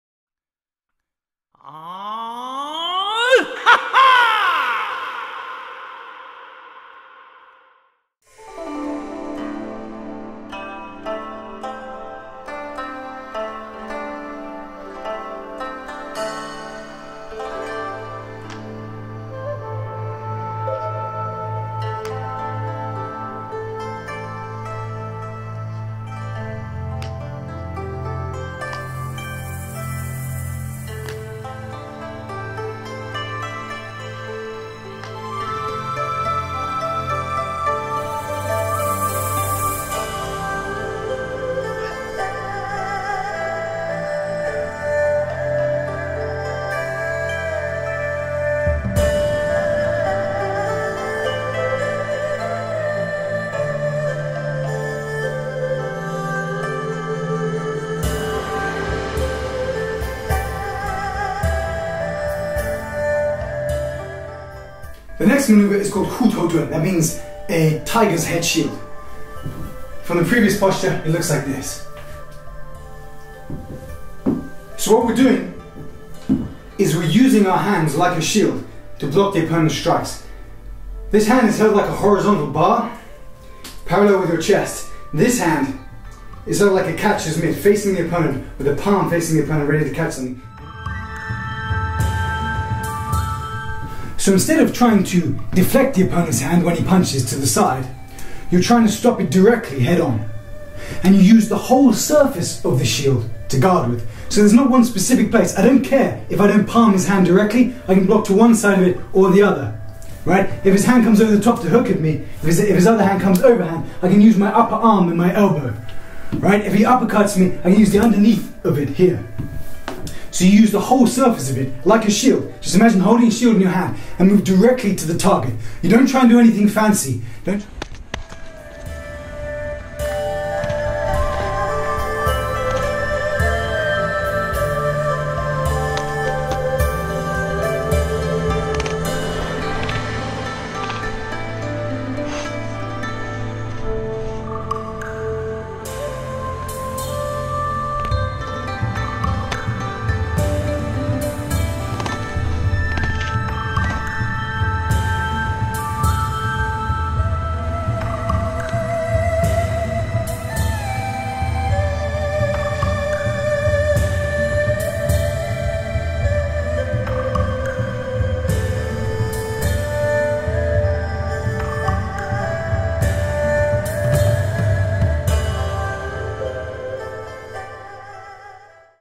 Shaolin 13 Hammers Full Kung Fu form tutorial - Trailer
A full instructional video for the Shaolin form '13 Hammers'. This is a basic form in Shaolin Kung fu. The form is only short but the instructional video is a full 2 hours teaching every movement of the form in detail as well as the applications.